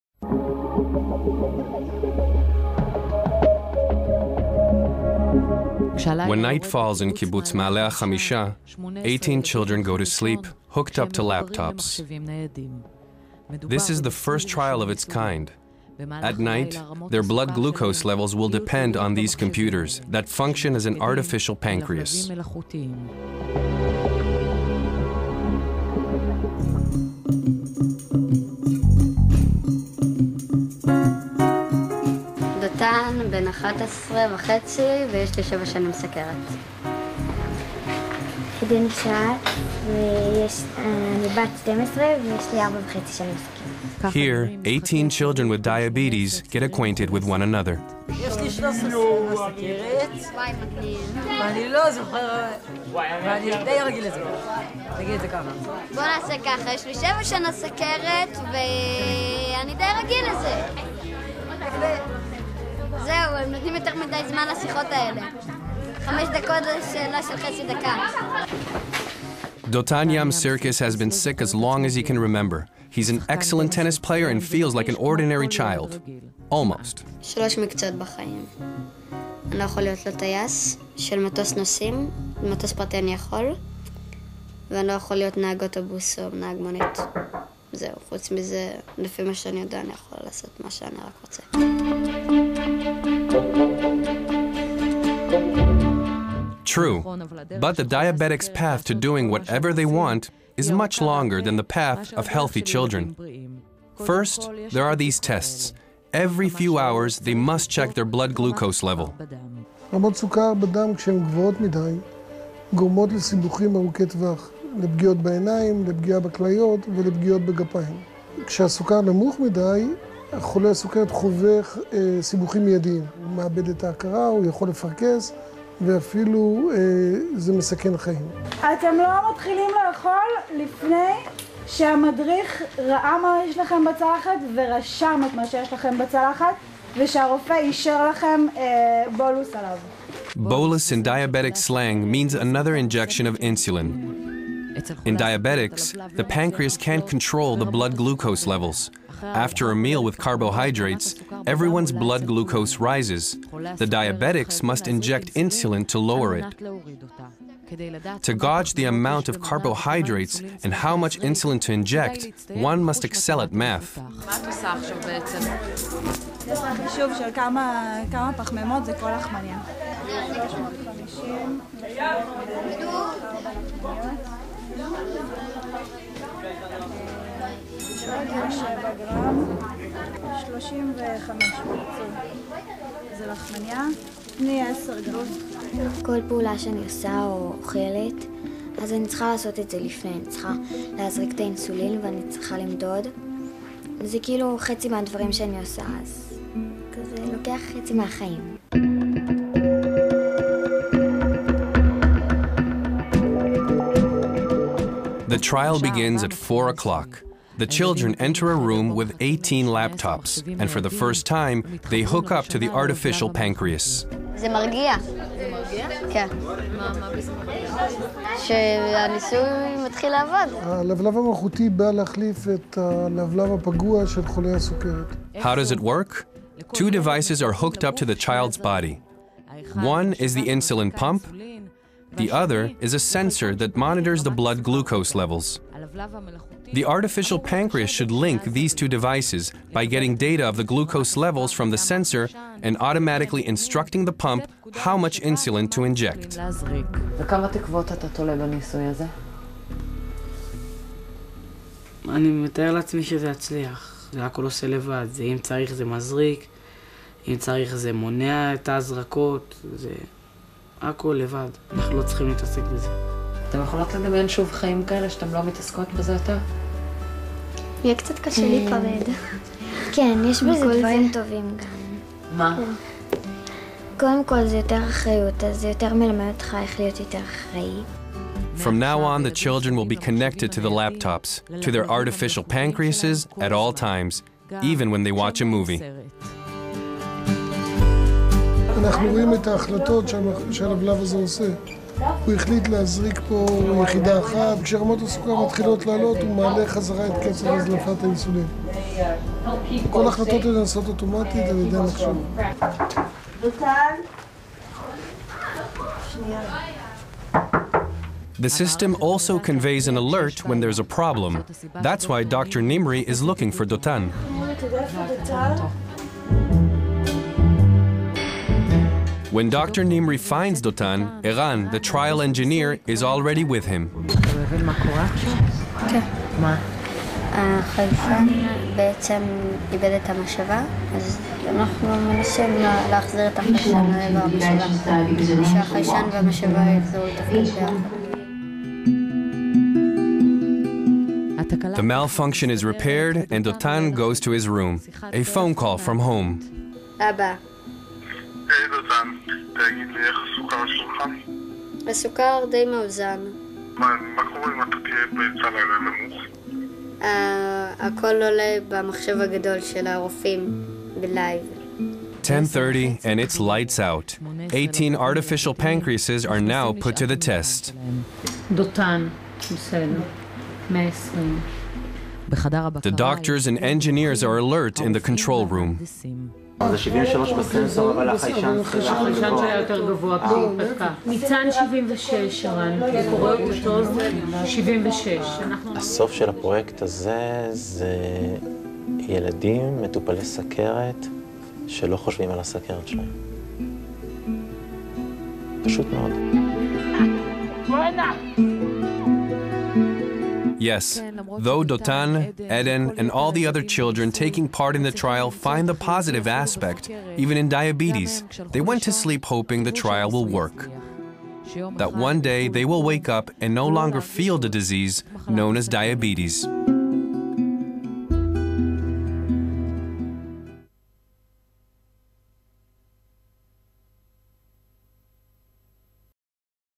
Must See: The first trial with Artificial Pancreas system in a diabetes camp - Voice over in English
The Diabetes wiREless Artificial pancreas consortiuM (DREAM) recently evaluated the MD-Logic Artificial Pancreas (MDLAP) system in an innovative trial project taken place in Israel, Slovenia and Germany. The aim of the trial was to provide a real solution to patients with diabetes, using the MDLAP in a diabetes camp outside the confines of the hospital. This is a significant landmark in research: this is to the best of our knowledge the first time in the world that such a trial project has been taken outside the hospital, illustrating a "normal" life for youngsters with diabetes, while using an automatic insulin delivery system during the night.

The MD Logic system which was developed at Schneider Children's represents the "artificial pancreas" technological solution comprising of an off-the-shelf subcutaneous glucose sensor that monitors the glucose level and an insulin pump. The sensor and pump are connected to a computer that programs the information and stipulates the amount of insulin that should be released to the body in order to maintain blood glucose balance. This innovation "closes the loop" between the sensor and the pump and relieves the patients with diabetes from the daily burden of dealing with their diabetes and has the potential to significantly improve the quality of life of patients with diabetes.
The artificial pancreas trial outside the hospital was conducted as a prospective cross-over study within the framework of the 3-day DREAM Camp for children with diabetes.

The movie shows the first trial conducted at the Kibbutz Maale Hachamisha Hotel, Israel. The study was consisted of 18 children and youth between the ages of 12-15 years stayed. Nine children were connected to the artificial pancreas system on the first night of the camp and eight children were connected on the second night. Monitoring the night-time glucose levels is extremely important since most cases of severe hypoglycemia occur during the night and blood glucose levels are not within the desired range in many of the patients while they are asleep. The team of engineers and medical staff stayed in the control room set up at the hotel on both nights, from where they were able to remotely supervise the trial and monitor the glucose levels of the children. Within the framework of the camp, social activities with counselors and the team of the Institute for Endocrinology and Diabetes took place: the children enjoyed the interaction between themselves, swimming in the pool, sports, watching movies and other fun activities.